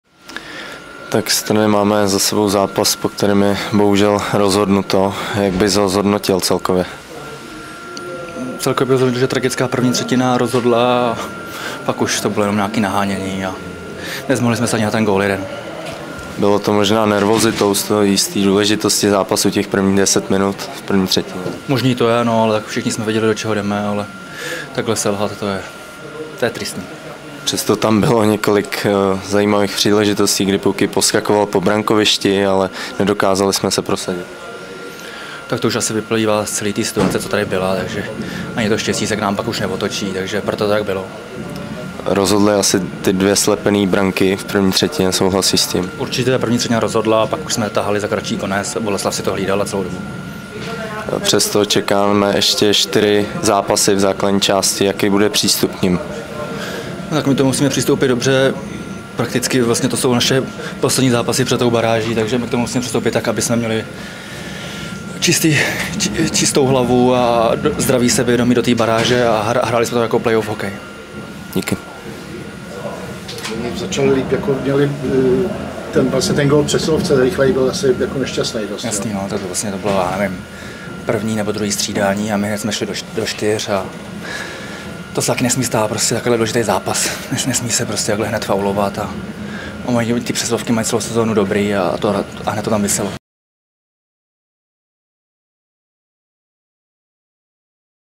Rozhodla tragická první třetina, komentuje nevydařený zápas Strnad
Poslední zápas naděje pro Kladeňáky nedopadl dle představ a po výsledku 0:5 si opět zahrají baráž: „Čekají nás teď čtyři utkání v základní části, ke kterým musíme přistoupit s maximální koncentrací na baráž," vystihuje aktuální situaci útočník Jakub Strnad.